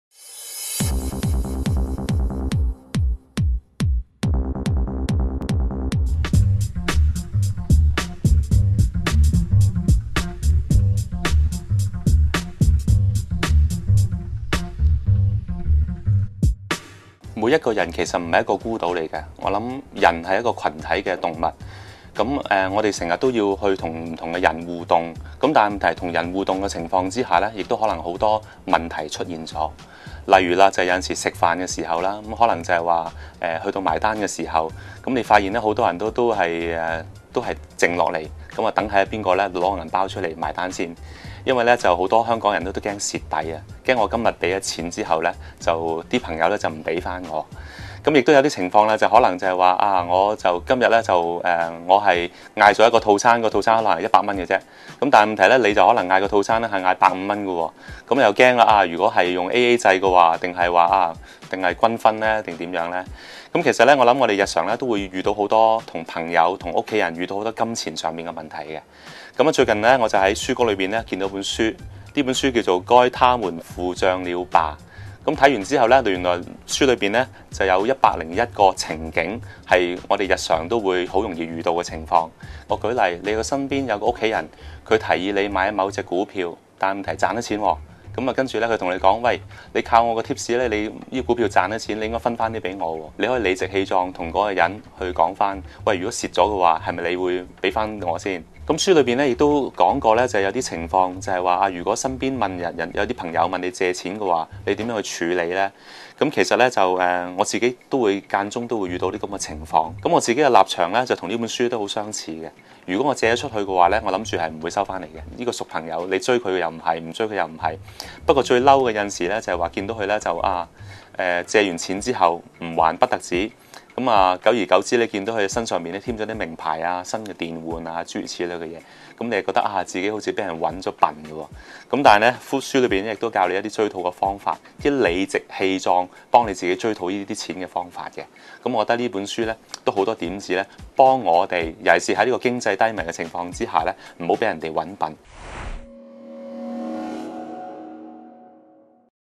《信報視聽頻道》 財經閱讀推介：該他們付帳了吧！‎
瀏覽專題特輯「財經閱讀」，收看更多好書推介

你都可能曾經遇過：投資股票賺了一大筆錢，不想讓家人知道；弟弟買了一輛新車，卻不還我借給他的錢；朋友身上總不帶錢，不管哪裡都要你付鈔；約會兩、三次，女朋友便查問我的財務狀況這些問題都很常見，也都很難處理，因為「怕講錢傷感情」、「不好意思拒絕」，不妨考慮作者提供周詳而且合乎人情義理的實用建議。
作者Jeanne Fleming和Leonard Schwarz是一對夫妻檔作家，目前是《錢》雜誌 (Money Magazine)「做對的事」(Do the Right Thing)專欄的共同作家。《錢》雜誌發行近200萬冊，在個人理財雜誌市場上頗具聲譽。此專欄同時也出現在 CNNMoney.com網站上。
主持戴永強，是跨國保險集團營業總監，仍然書不離手，具備十多年銷售及管理經驗，讀每本書都是智慧和實戰經驗的結合。